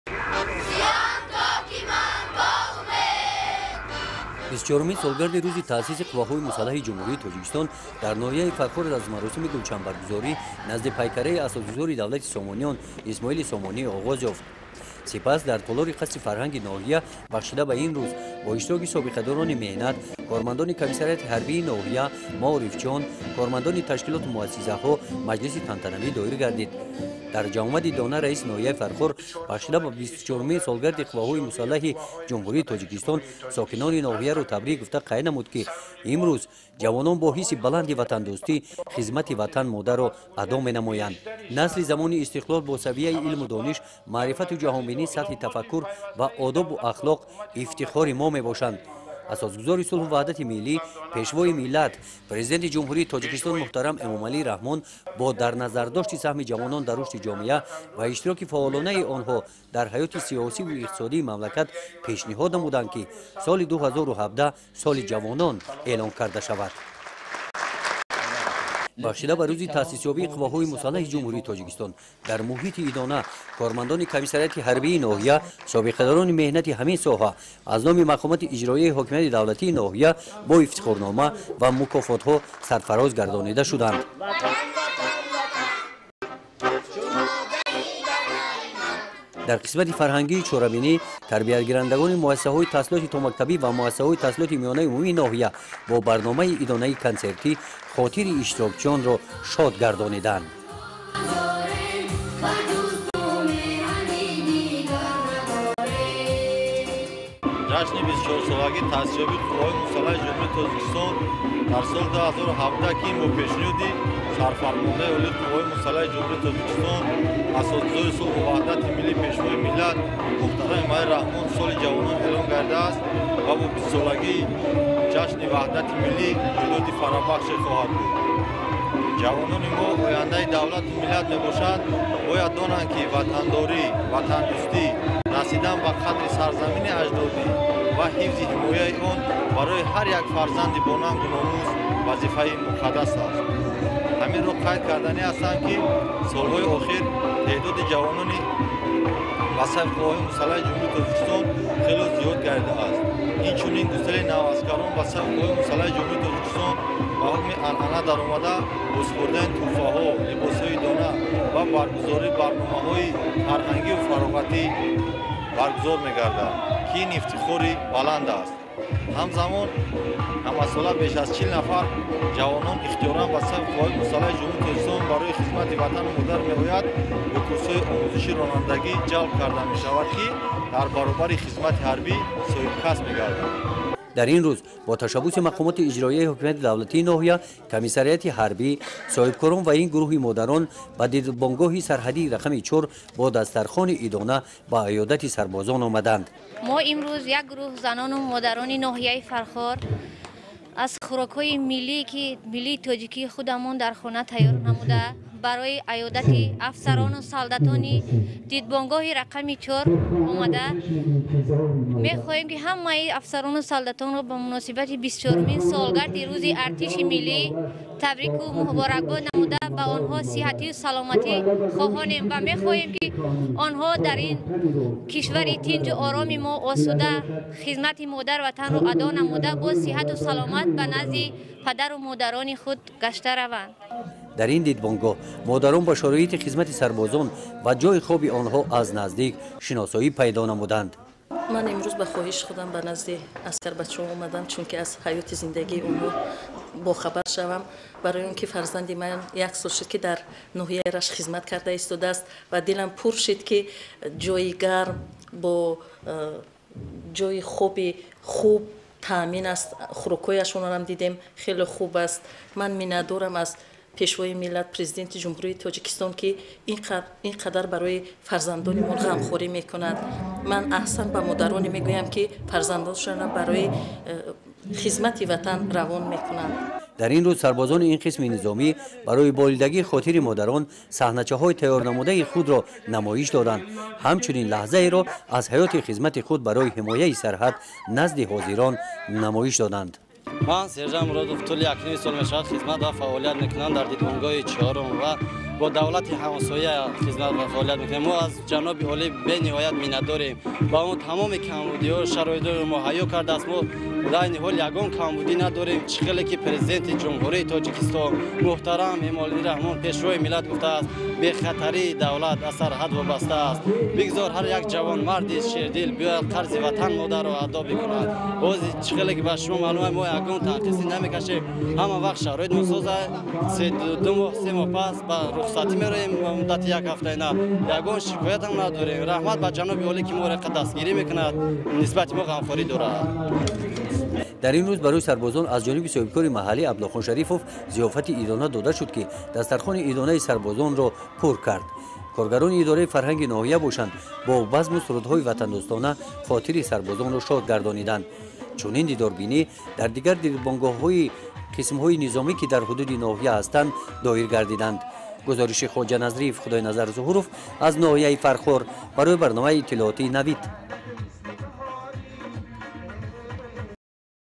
Рузи Артиш дар Фархор
Бахшида ба 24 умин солгарди рузи таъсиси куввахои мусалахи Чумхурии Точикистон дар нохияи Фархор чорабинихои идона доир гардидаанд. Дар ин руз модарон ба дидорбинии  сарбозон  омаданд.